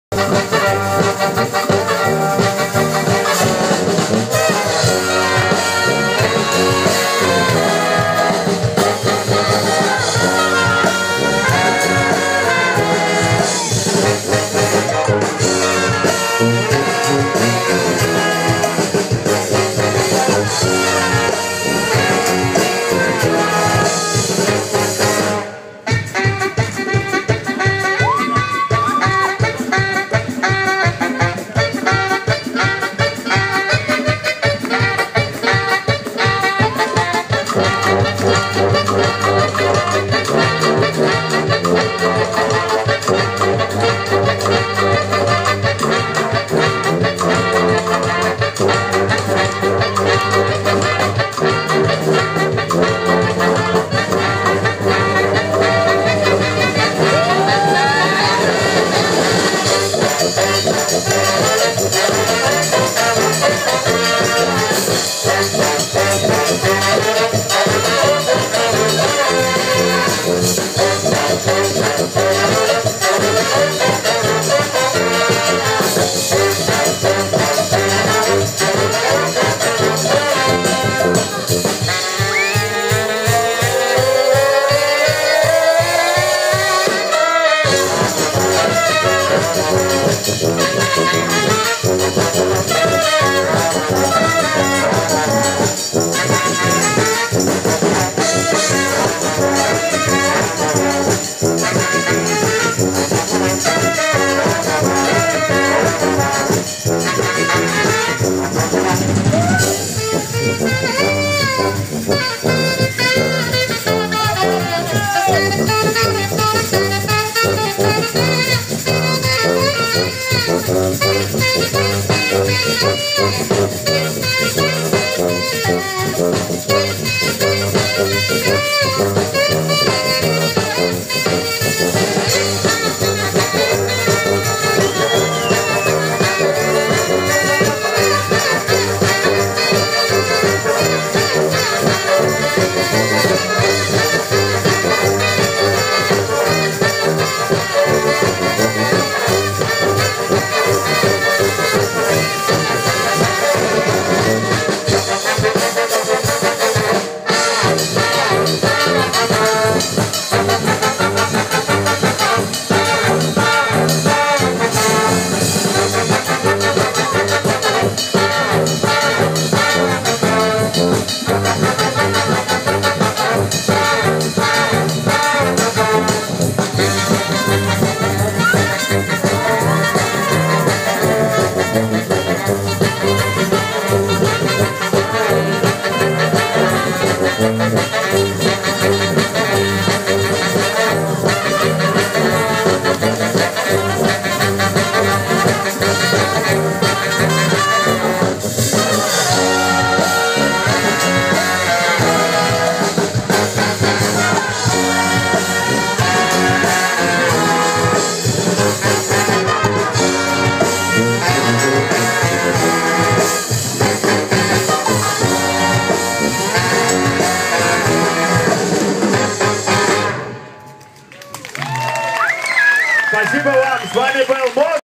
MosBras (МОСБРАС)
Last song on their Usadba Jazz 2013 set.